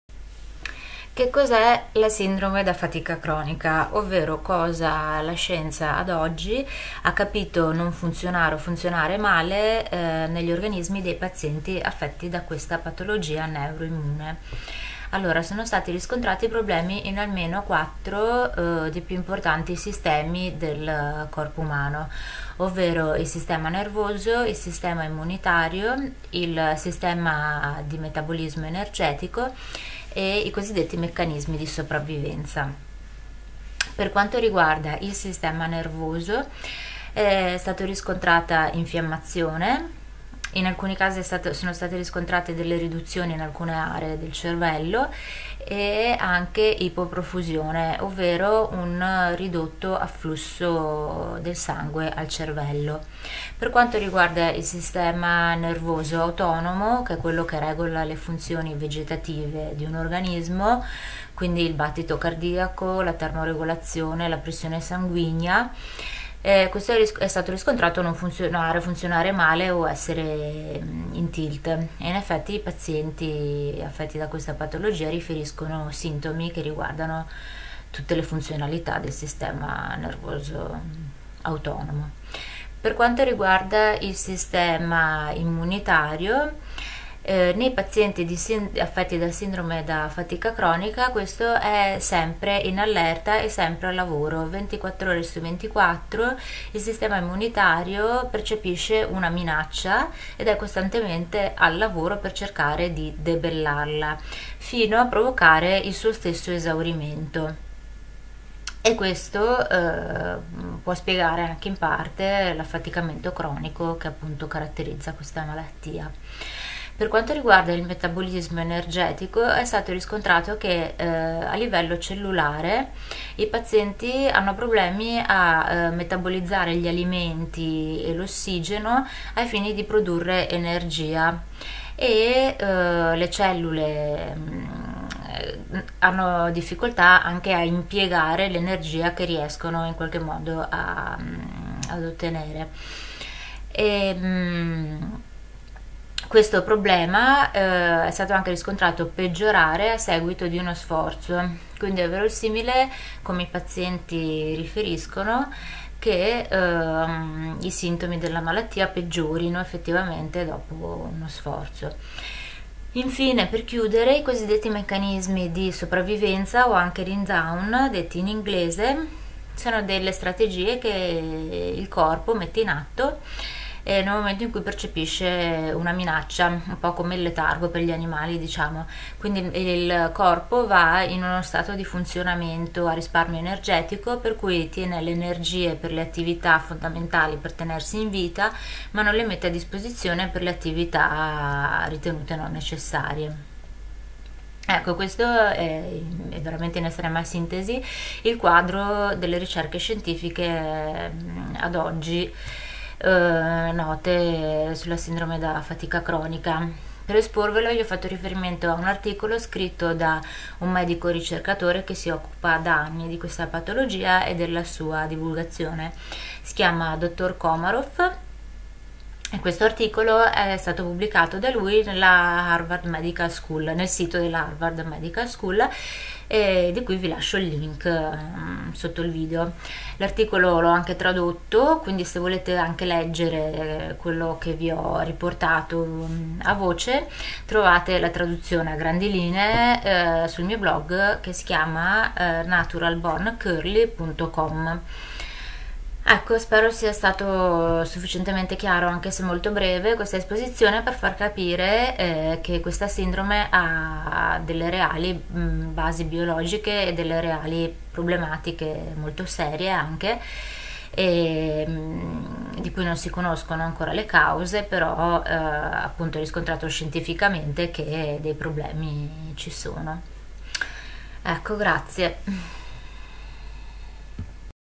Che cos'è la Sindrome da Fatica Cronica
Cosa sa la scienza ad oggi della Sindrome da Fatica Cronica o Encefalomielite Mialgica o ME/CFS, tratto dall'articolo del Dr. Komaroff di cui trovate traduzione sul mio blog: